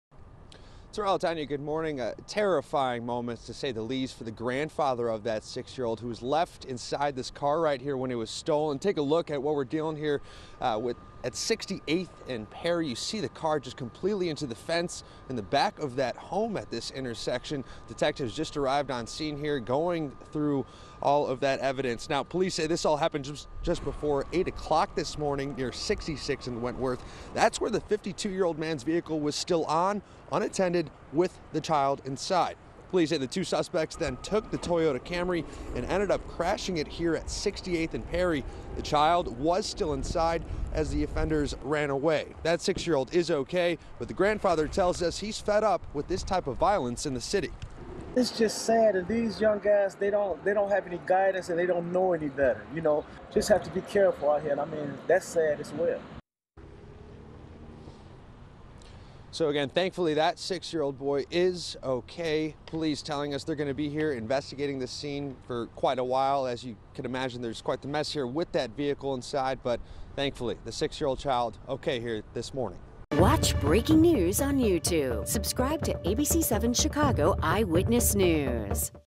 Car stolen with boy , 6, inside in Chicago
Chicago police said thieves stole a car with a six-year-old child inside in the Englewood neighborhood Thursday morning.